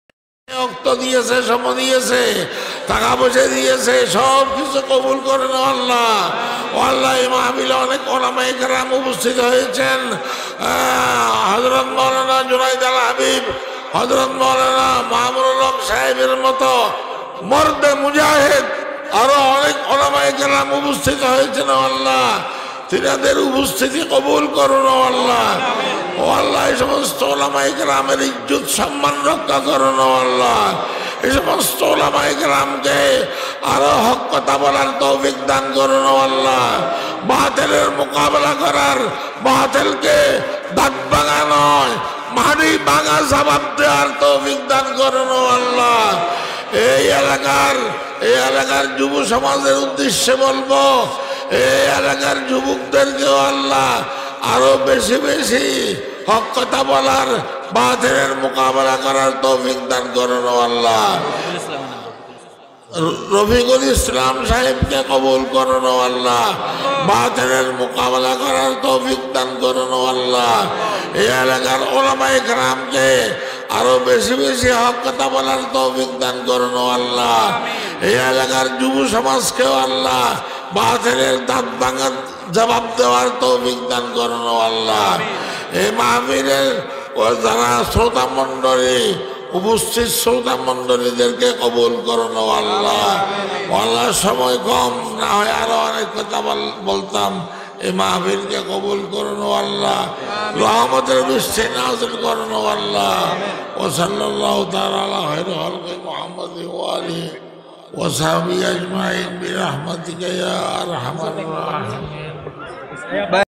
আল্লামা মামুনুল হক ।। জুনায়েদ আল হাবিব ।। রফিকুল ইসলাম মাদানী সহ সবার জন্য দোয়া করলেন বাবুনগরী হুজুর
Rowja Islamic Tv চ্যানেলে আপনাকে স্বাগতম

এই ভিডিওতে জুনায়িদ বাবুনগরী হুজুর আল্লামা মামুনুল হক জুনায়েদ আল হাবীব রফিকুল ইসলাম মাদানী সহ সকল আলেমদের জন্য দোয়া করেছেন এই মাহফিলটি নেত্রকোনার ২০২১ সালের আরো নতুন নতুন বাংলা ওয়াজ পেতে আমাদের সাথেই থাকুন ।